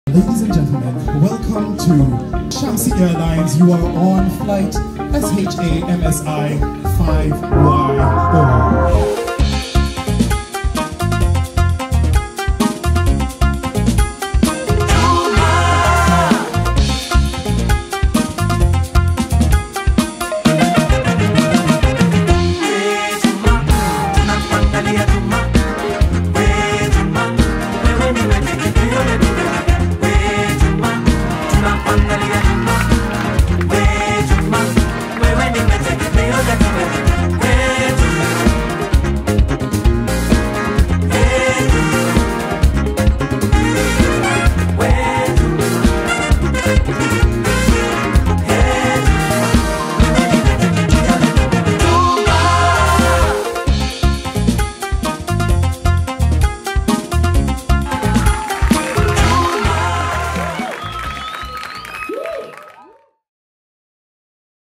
Shamsi Fest Highlights
Shamsi Music turns 5! Enjoy some moments as captured during the Birthday Bash on 10th November, 2019.